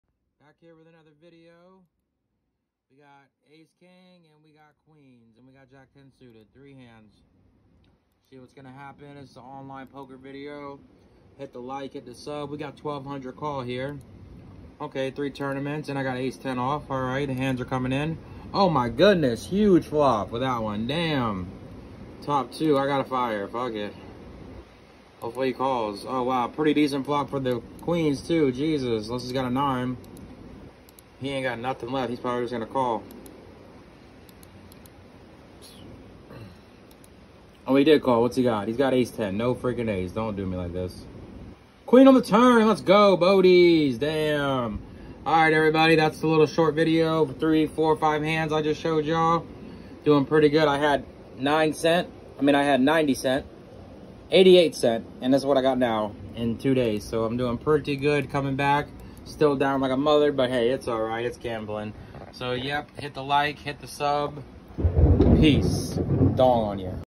Online Poker Video showing off 4 hands won them all lego DONGONYA had 88 cent now over 100
Like and sub for more online poker replays